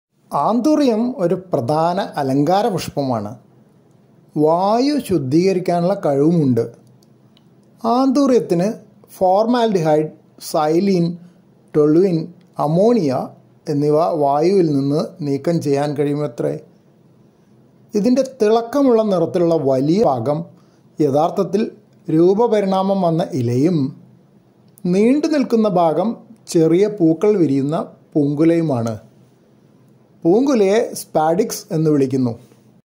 ചുവന്ന ആന്തൂറിയം പുഷ്പം
ആന്തൂറിയം ഒരു പ്രധാന അലങ്കാര പുഷ്പമാണ്. വായു ശുദ്ധീകരിക്കാനുള്ള കഴിവുമുണ്ട്. ആന്തൂറിയത്തിന് formaldehyde, xylene, toluene, ammonia എന്നിവ വായുവിൽ നിന്ന് നീക്കം ചെയ്യാൻ കഴിയുമെത്രെ. ഇതിന്റെ തിളക്കമുള്ള നിറത്തിലുള്ള വലിയ ഭാഗം യഥാർത്ഥത്തിൽ രൂപപരിണാമം വന്ന ഇലയും നീണ്ടുനിൽക്കുന്ന ഭാഗം ചെറിയ പൂക്കൾ വിരിയുന്ന പൂങ്കുലയുമാണ്. പൂങ്കുലയെ സ്പാഡിക്സ് എന്ന് വിളിക്കുന്നു.
Anthurium andraeanum can remove formaldehyde, xylene, toluene, and ammonia from the air. It is an important ornamental flower. The large brightly coloured part is actually a spathe leaf and the protruding part is the inflorescence which bears tiny flowers. The inflorescence is called spadix. Web